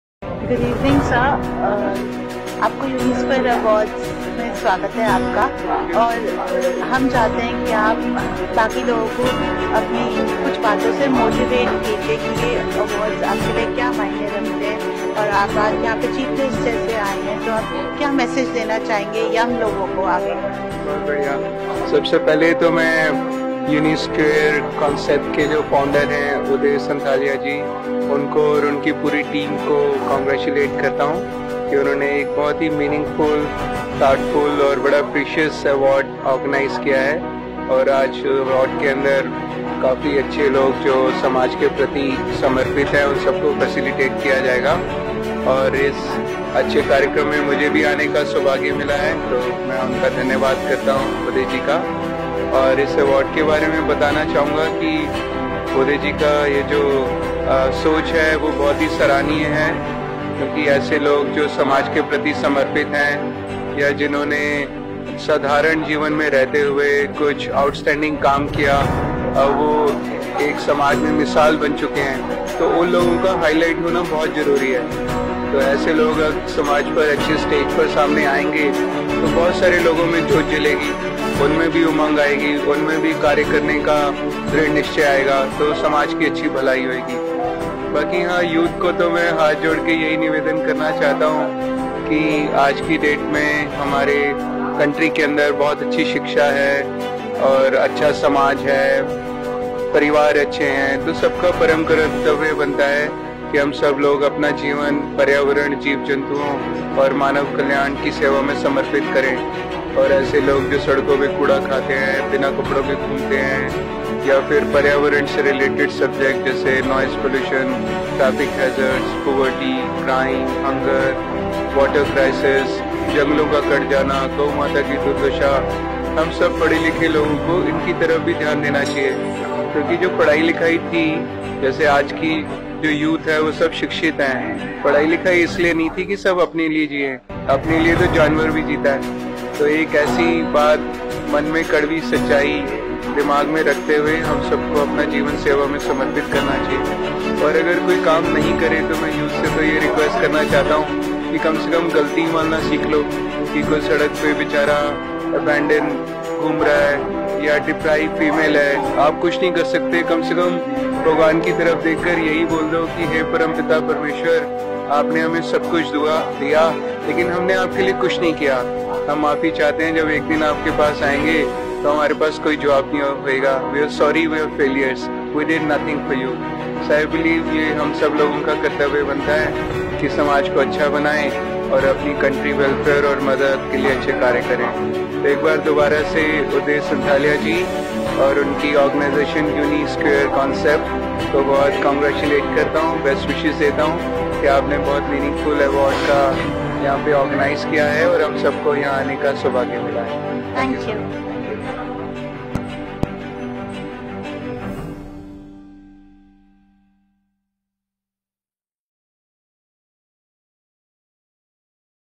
Shri Ravi Kalra on Uni Square Concepts at The Uni Square Awards 2019
Karam Yogi Shri Ravi Kalra, Founder & President of The Earth Saviors Foundation, sharing his views about our CEO Uday Sonthalia and Uni Square Concepts at The Uni Square Awards 2019.